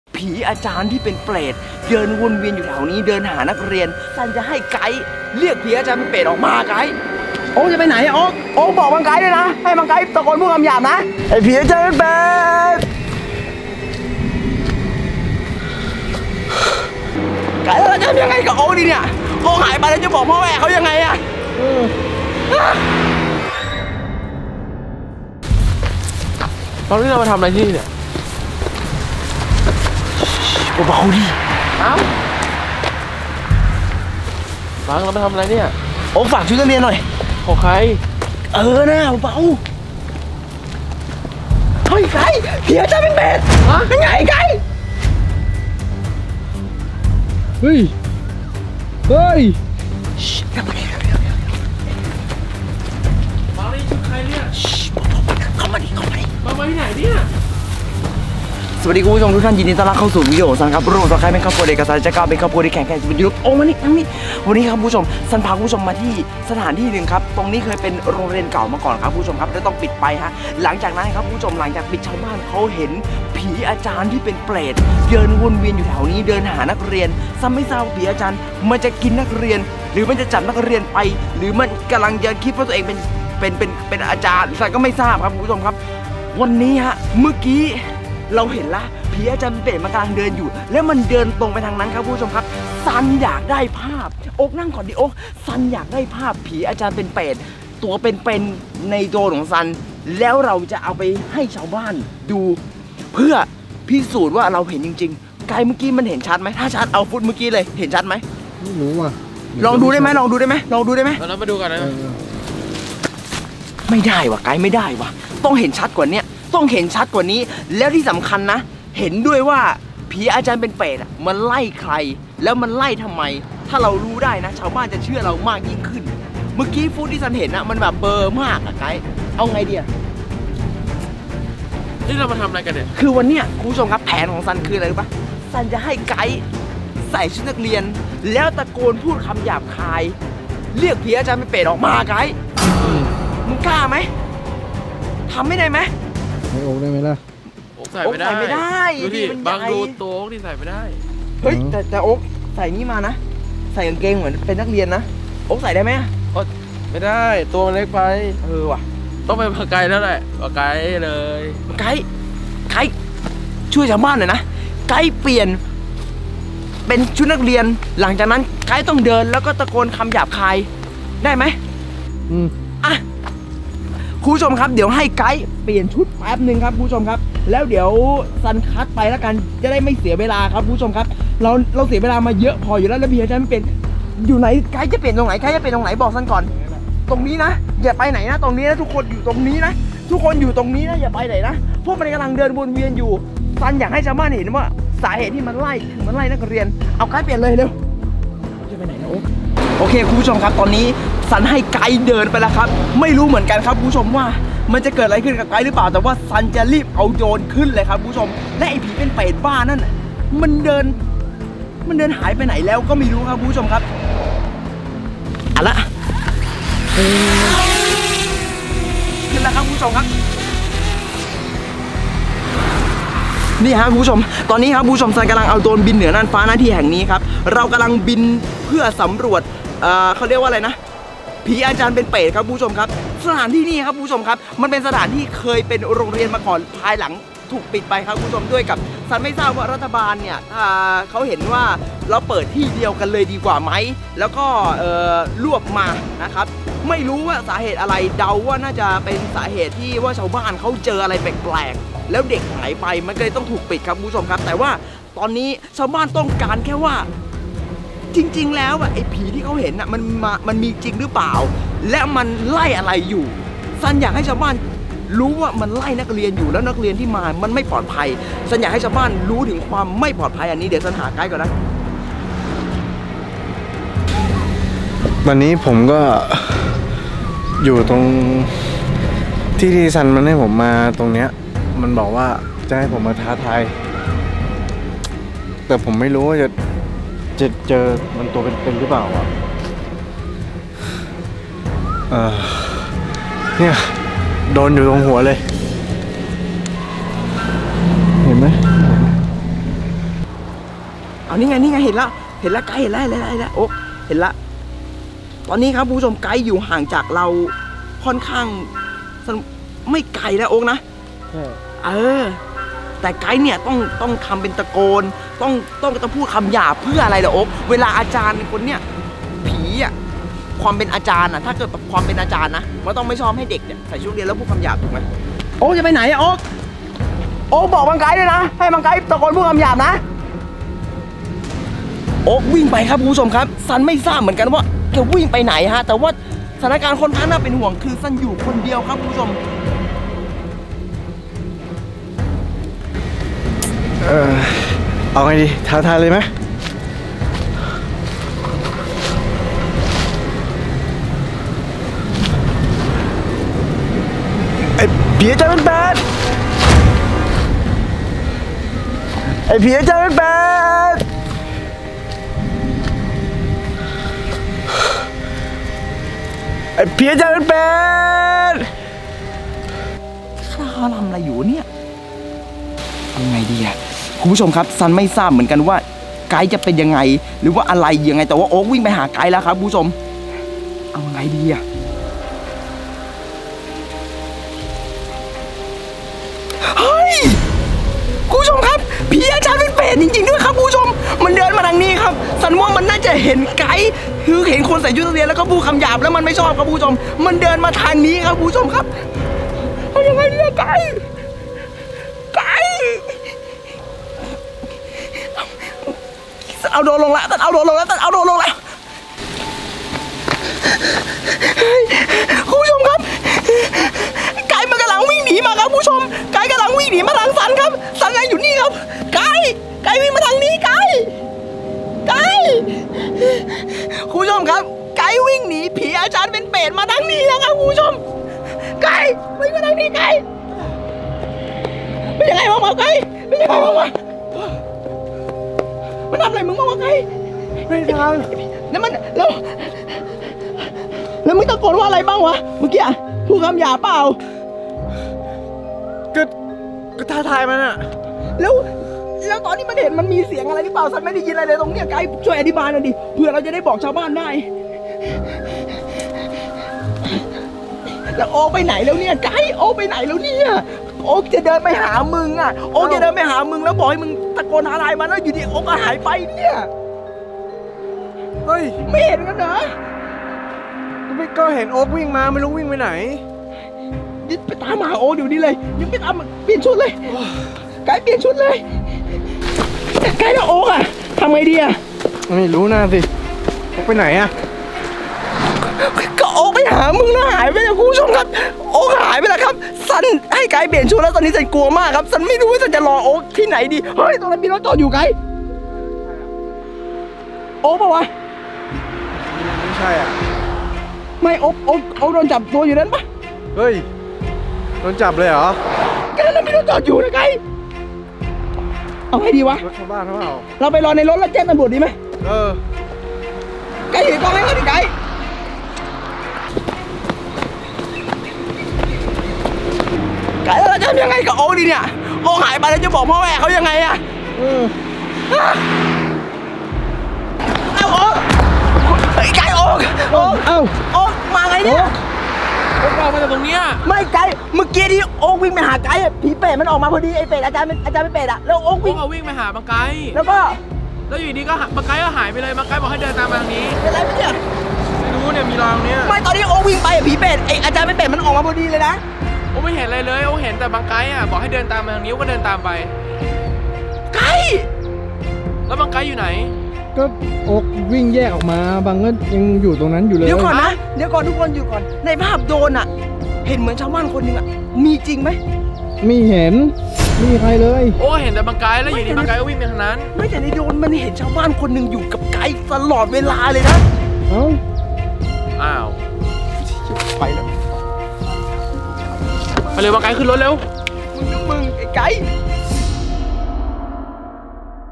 ให้เพื่อนใส่ชุดนักเรียนตะโกนด่าคำหยาบ! ท้าทายผีอาจารย์เป็นเปรต |โดรนจับภาพEp114
ช่องเด็กฝึกงานโอ๊ค : Thep Oak